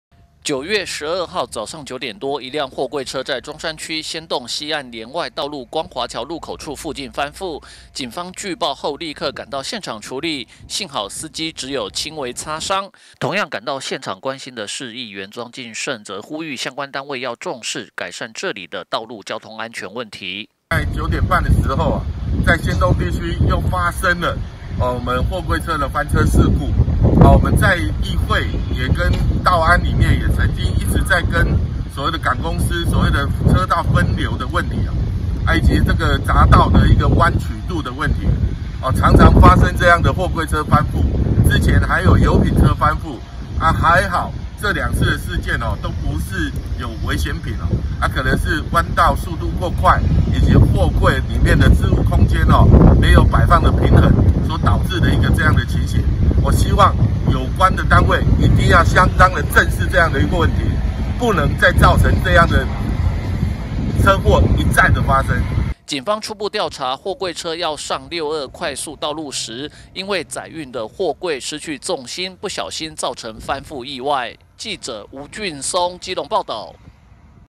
新聞 基隆|基隆交通意外|西岸聯外出入口前貨櫃車翻覆|基隆中山區仙洞貨櫃車翻覆|光華橋前貨櫃車翻覆 莊敬聖盼相關單位重視
光華橋前貨櫃車翻覆 莊敬聖盼相關單位重視

(中嘉吉隆新聞記者吳俊松/基隆報導)12號早上一輛貨櫃車在中山區仙洞西岸聯外道路光華橋入口處附近翻覆，警方據報立刻趕到現場處理，幸好司機只有輕微擦傷，同樣趕到現場關心的市議員莊敬聖則呼籲相關單位要重視，改善這裡的道路交通安全問題！